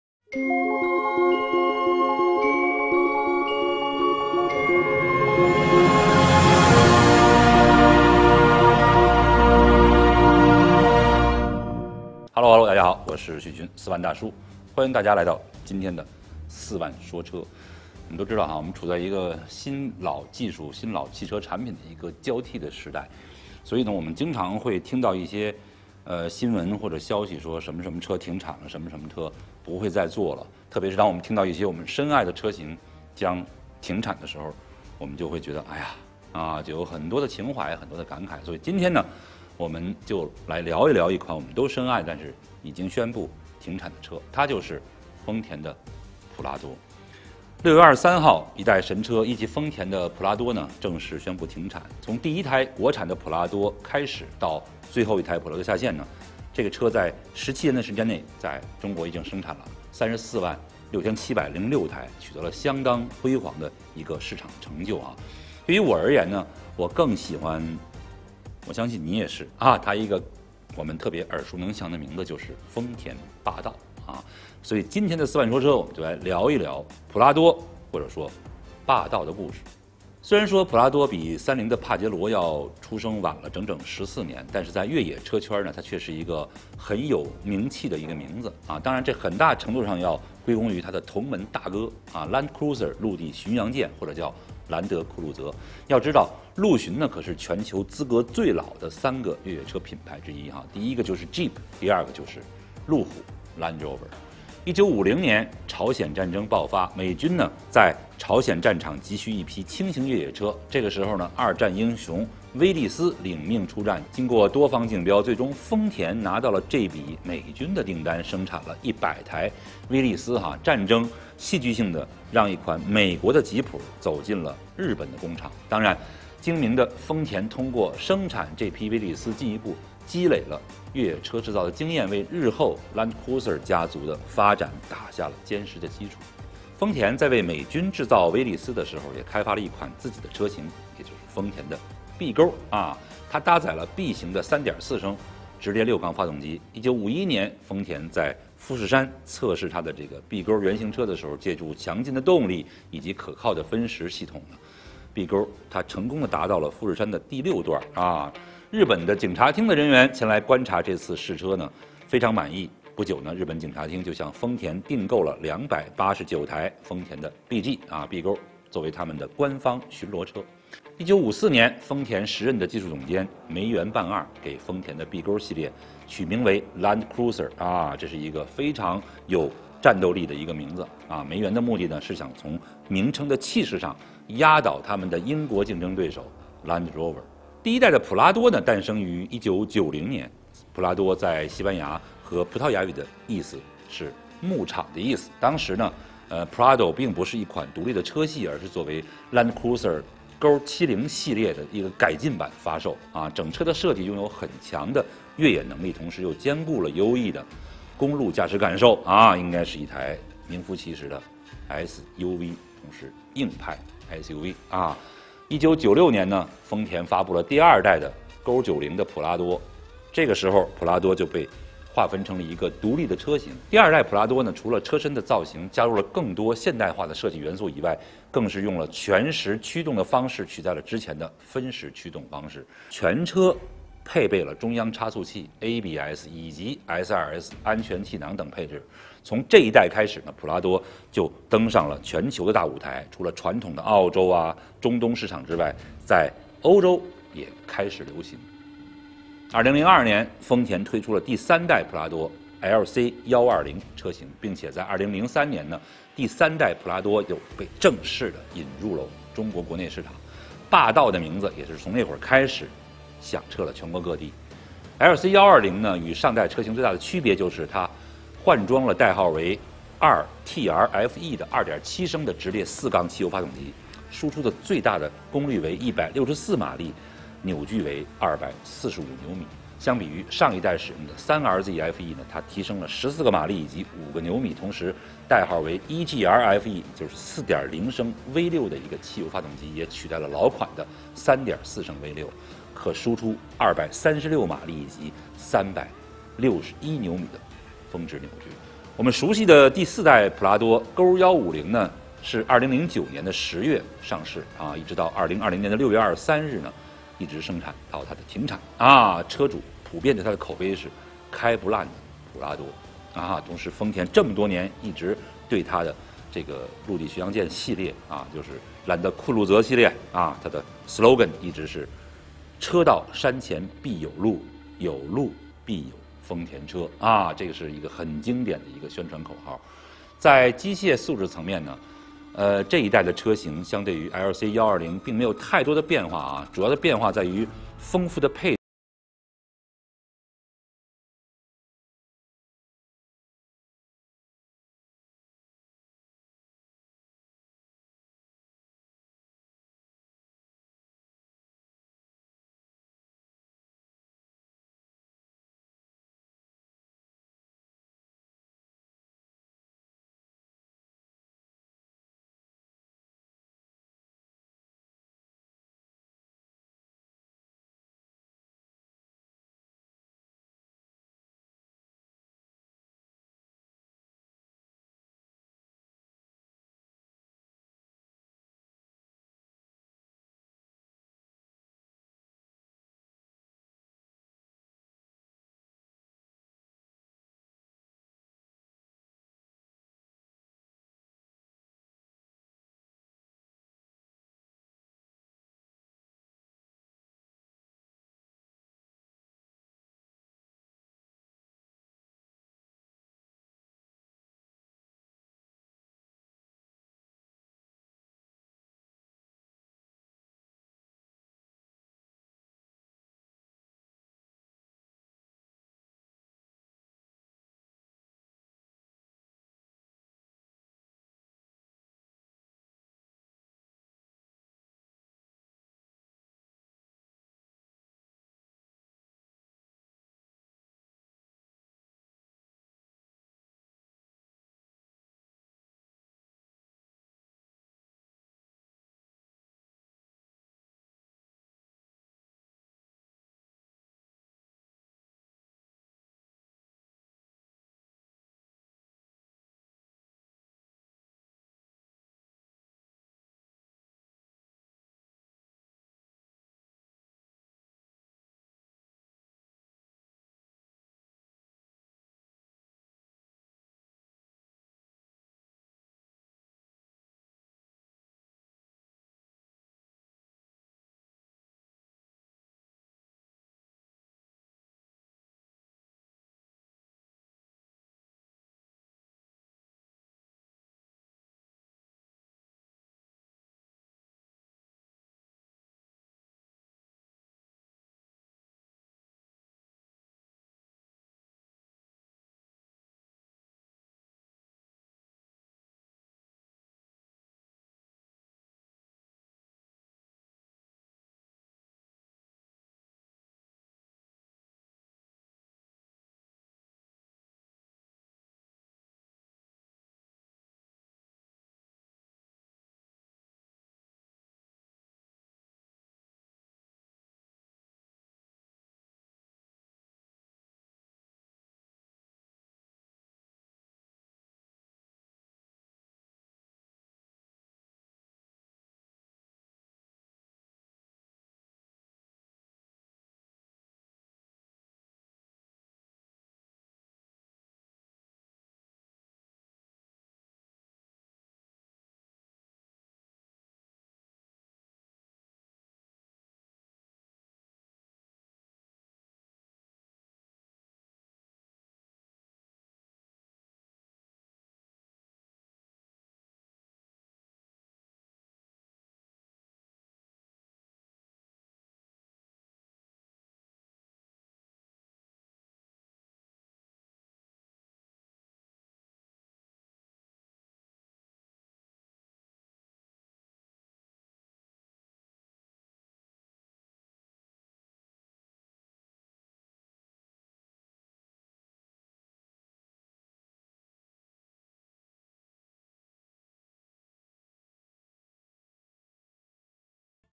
【四万说车】之丰田普拉多Prado的霸道人生
_________________★独家授权★_________________
6月23日，一代神车一汽丰田普拉多正式停产。从第一台国产普拉多开始生产到最后一台普拉多下线，该车型在这17年中总共生产了346706台，取得了相当辉煌的成就。对于我而言，更喜欢叫它另外一个耳熟能详的名字——霸道。时至今日，普拉多共经历四次换代，在经过接近三十年的历史积淀后，其硬件配置不断提升优化，舒适性不断提高。到了如今的第四代车型，普拉多已经不单纯只是作为越野车的存在，而是对于喜爱越野又需要城市驾驶需求的好伙伴。在日趋严格的排放标准下，大排量越野车逐渐离我们远去，它们会以什么姿态回到我们身边，我们不可预测。但是普拉多留给我们的精神和信赖，将会永远传递下去。